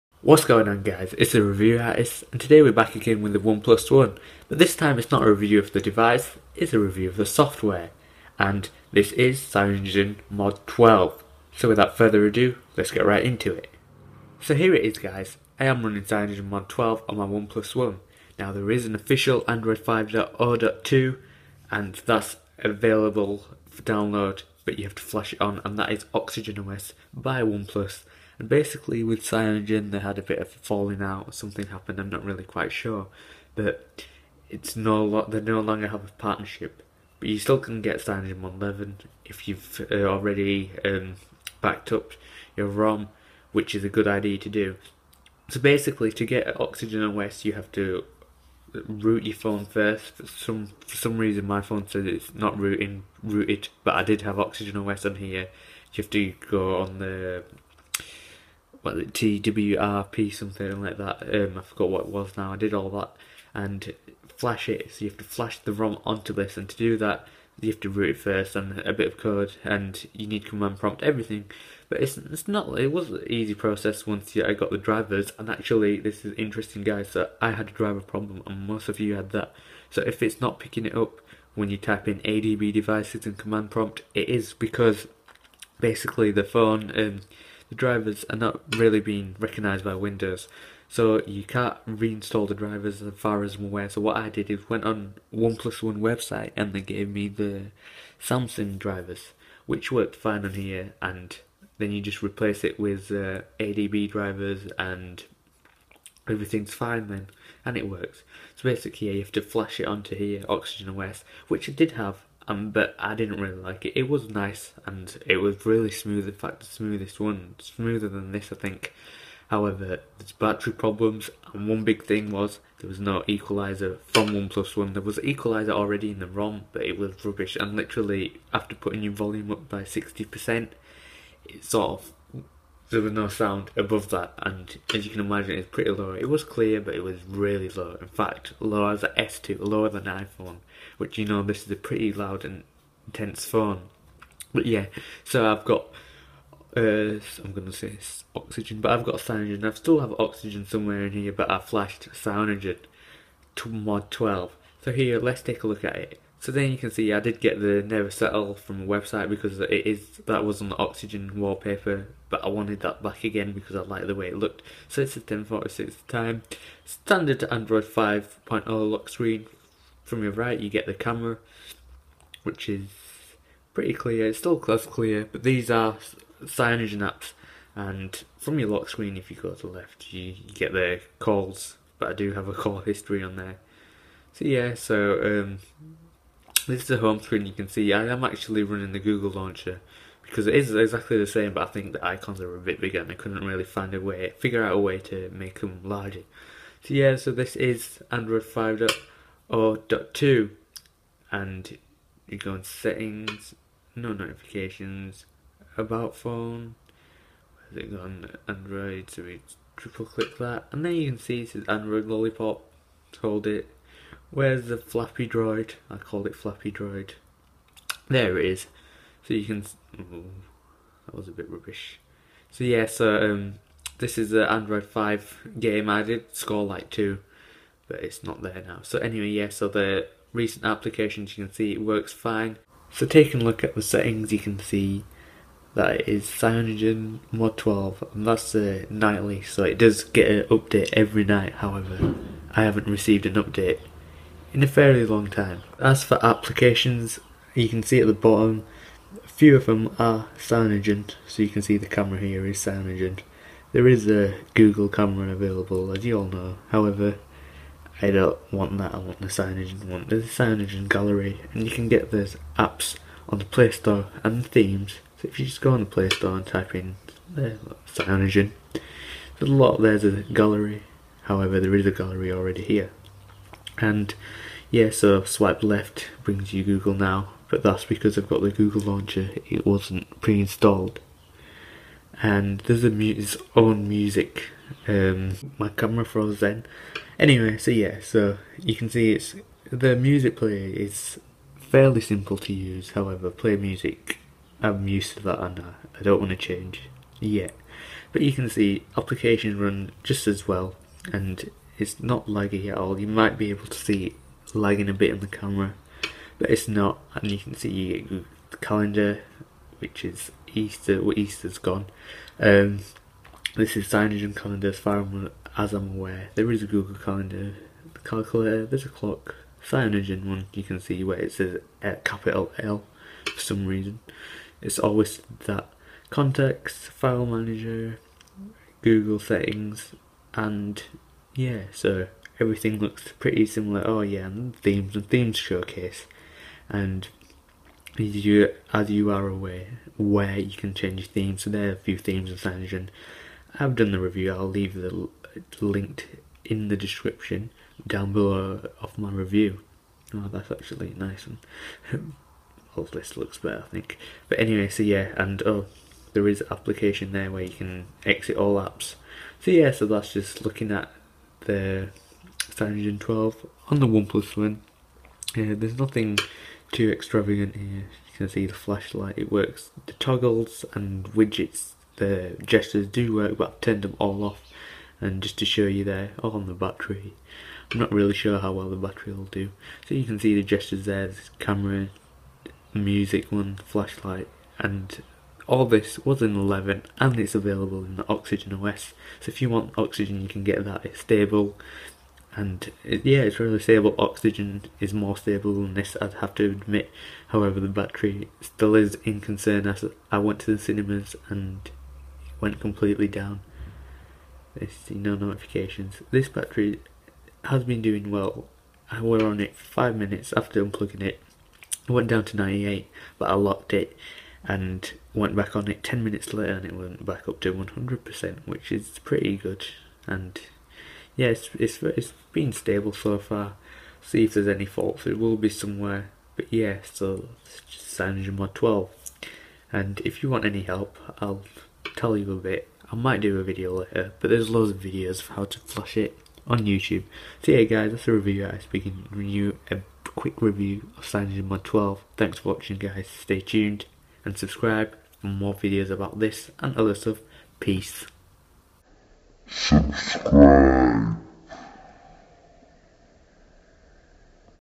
CyanogenMod 12 + OnePlus One
CyanogenMod 12 (nighly) running on the OnePlus One, after running OXYGEN OS I found a few issues so I flashed the CyanogenMod 12 ROM.

ROOT IS REQUIRED